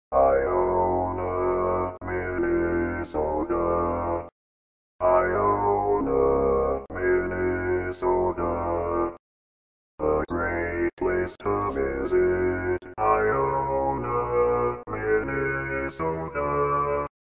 How to Say or Pronounce USA Cities — Iona, Minnesota
This video shows you how to say or pronounce Iona, Minnesota.

A computer said Iona, Minnesota. How would you say Iona, Minnesota?